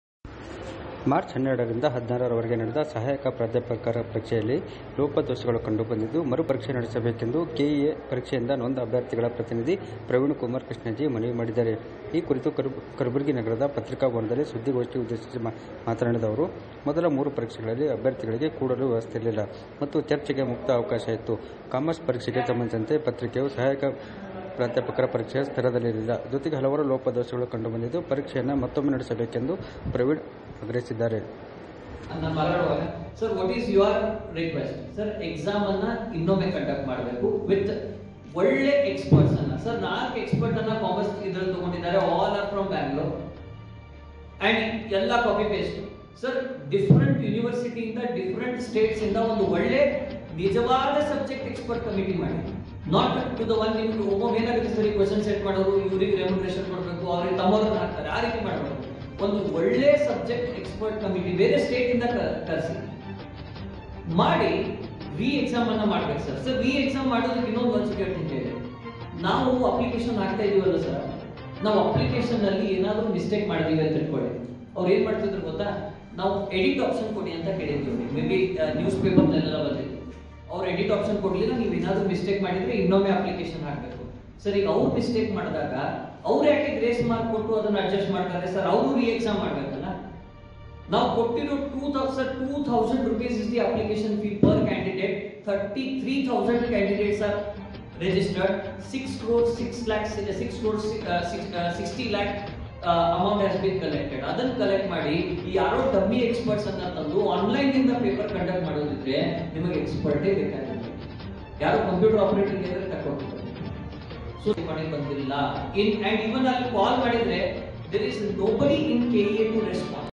KEA Examination irregularities, Press conference demanding re examination
KEA, Commerce, Assistant professor